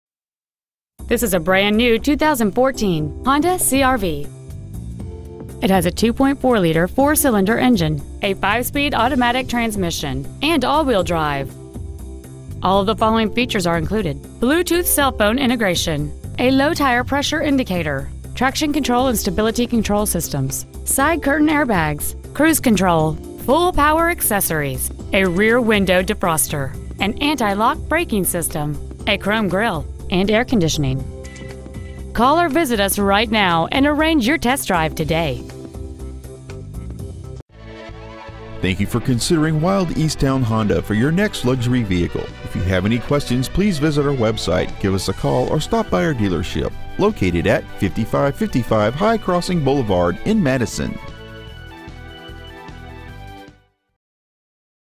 2014 Honda CR-V Madison WI
We have been honored to serve the Madison WI area , we promise that your experience at our dealership will exceed your expectations ! Year : 2014 Make : Honda Model : CR-V Engine : 2.4L 4 cyls Trans . : Automatic 5-Speed Exterior : Mountain Air Metallic Miles : 10 Interior : Black w/Cloth Seat Trim Stock : 13263 Wilde East Towne Honda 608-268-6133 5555 High Crossing Blvd.